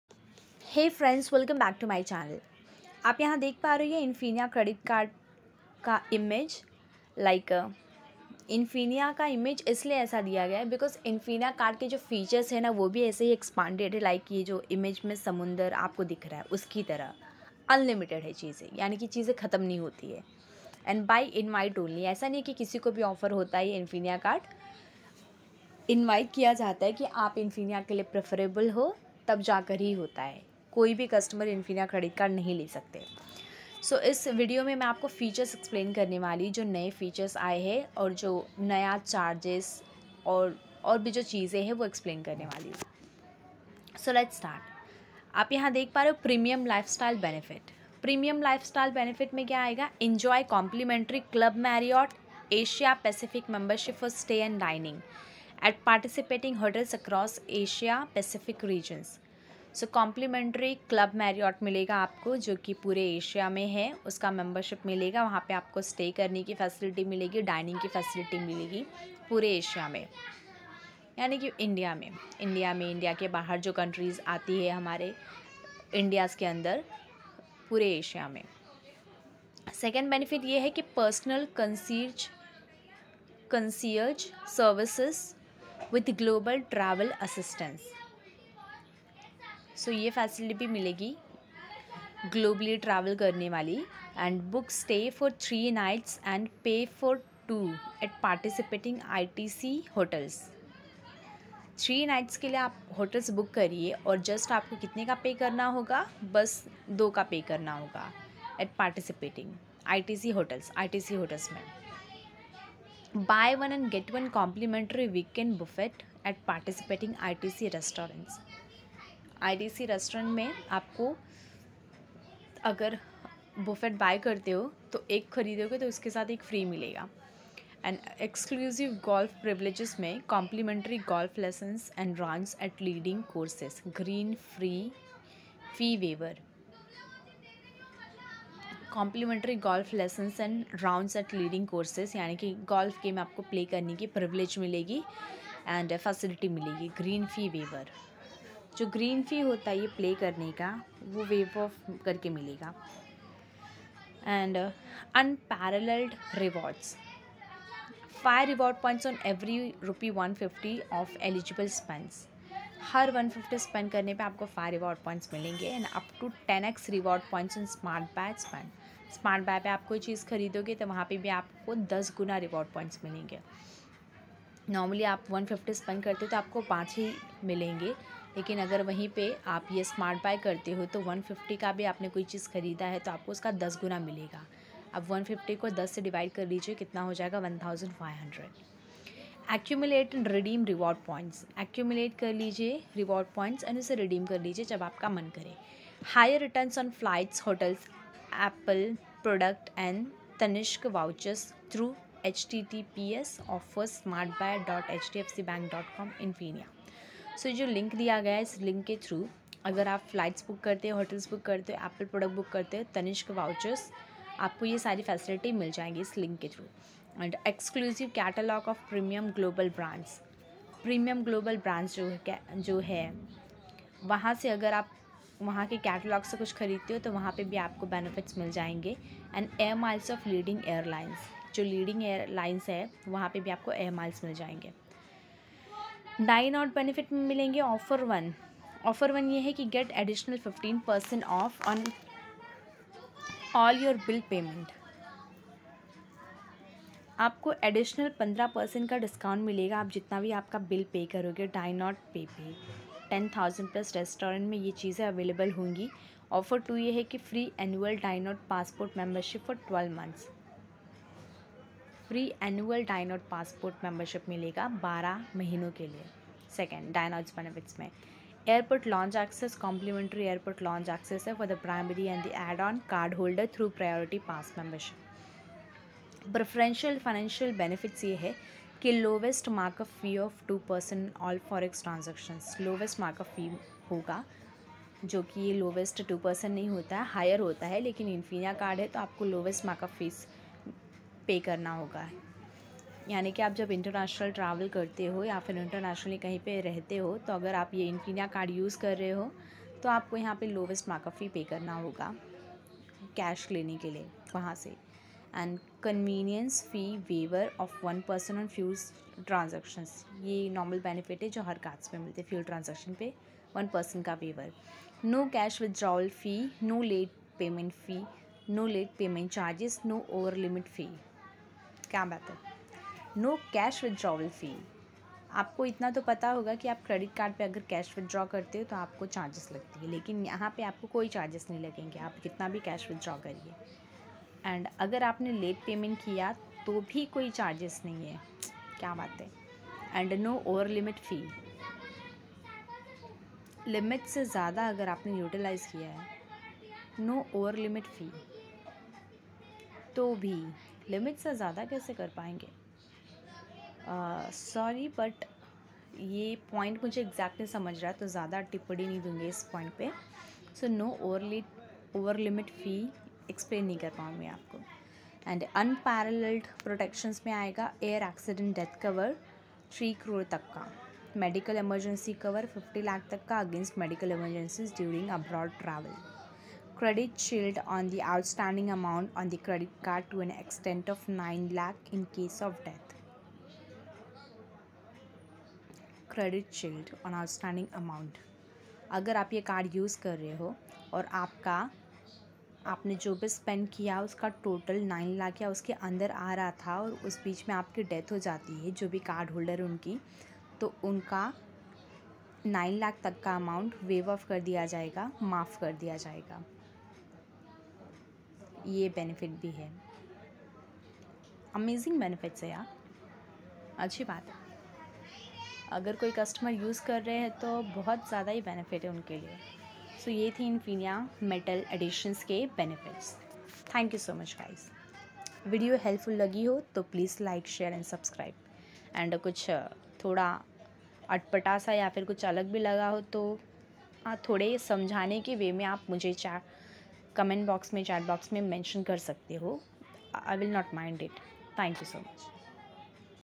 All New Features of INFINIA METAL EDITION OF HDFC Bank.
all features of infinia metal edition mention here and explained as well in short
truely it is a good card for higher spenders.